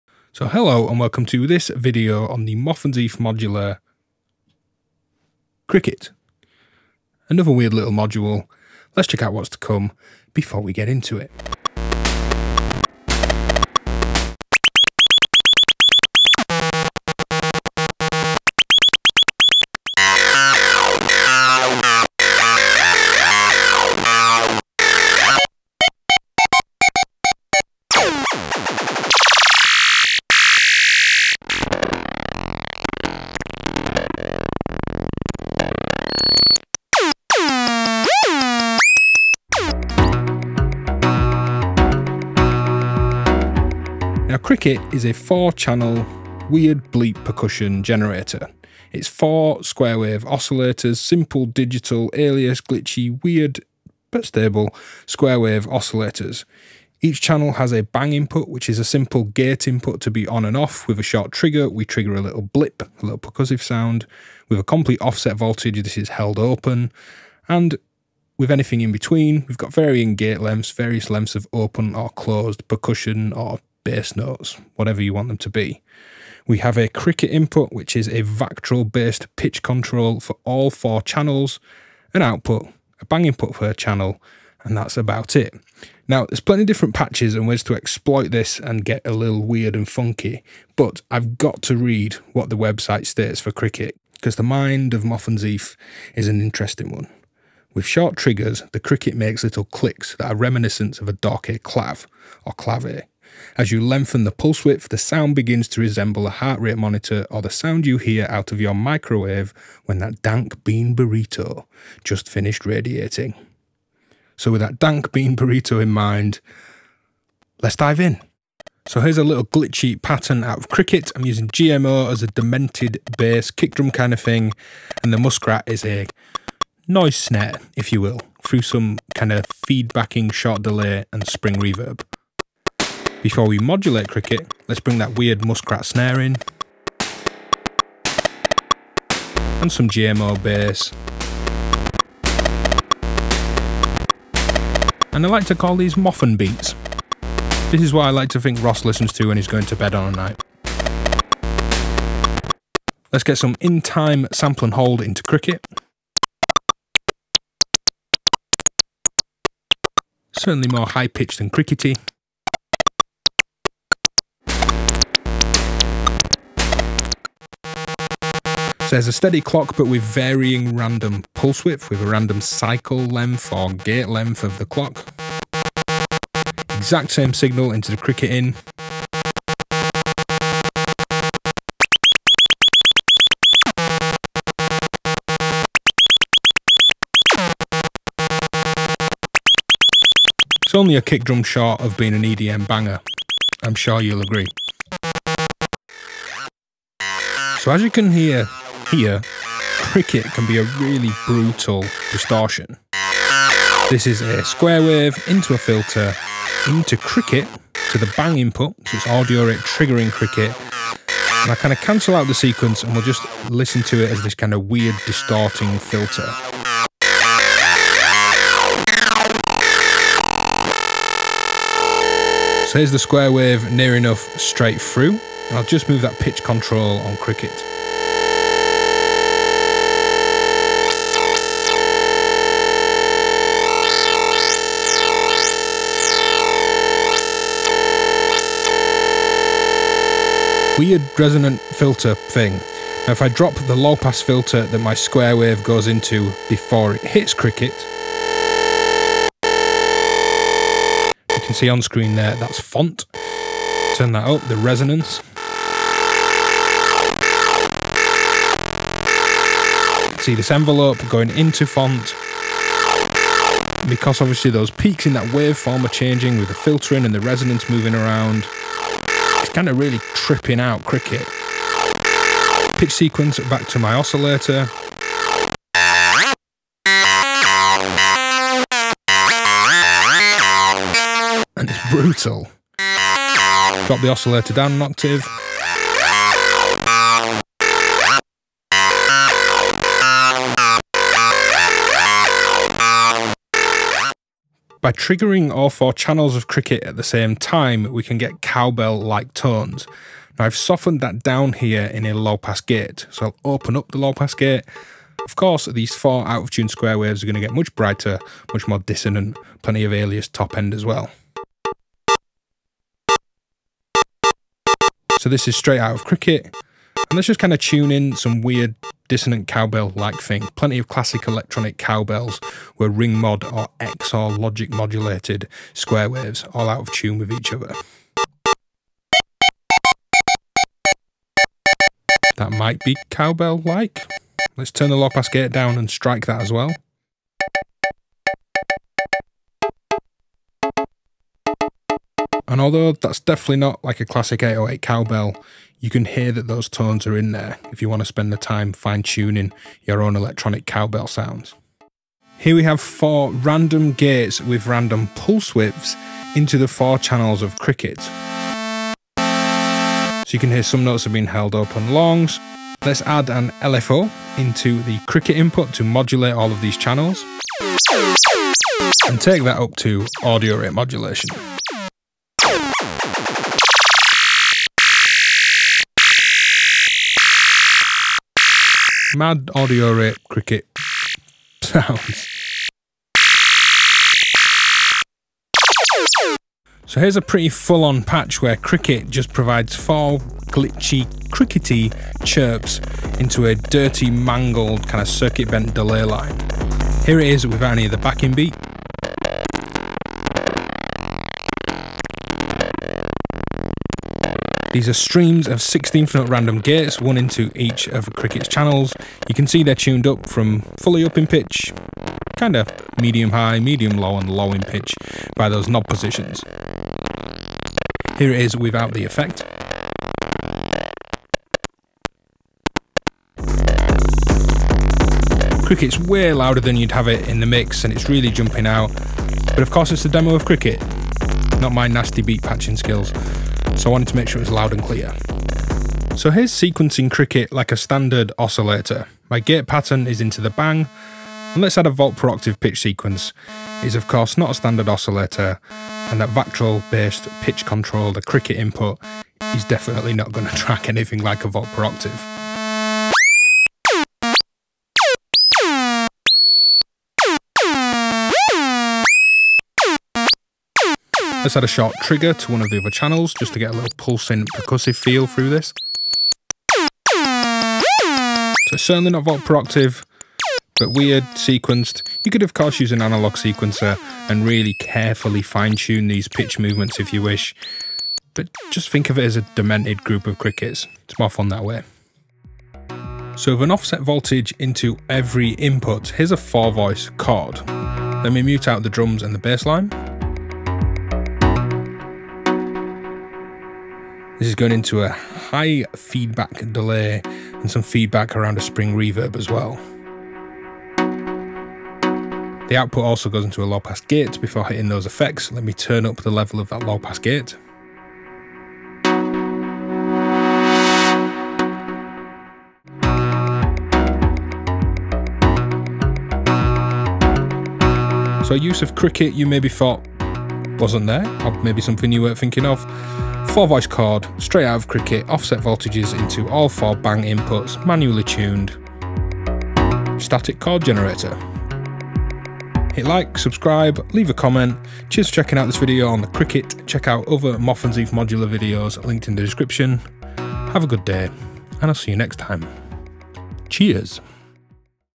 Moffenzeef Modular Kriket // Brutal Distortion, Beeps, Percussion, Heart Rate Monitor and more!
**TIMING INDEX / SECTIONS BELOW** Here we have the nasty little quad chirper KRIKET from Moffenzeef Modular. It's a four part bleep and blip percussion generator that mixes down square waves to it's main output. Each channel has a bang input (for gate) that responds to anything from triggers, varying gate lengths to offset voltages. You then get pitch control on each channel and a Kriket input which is a vactrol controlled pitch control. It gets weird and funky, but that's how we like it from Moffenzeef.

TIMING INDEX / SECTIONS

00:00 hello + video previews

00:43 WTF is Kriket?

01:58 Moffenbeat - beat with Dial Up, Muskrat + Kriket

02:47 Laser Krikets - how to make them and your own EDM laser insect rave

03:16 BRUTAL DISTORTION - processing sound through Kriket!!!

04:54 Cowbell synthesis - stacking square waves for primitive electronic percussion

05:52 Note length + audio rate modulation

06:26 Kriket excited FX lines

07:17 Sequenced behaviour

08:03 Static chord generator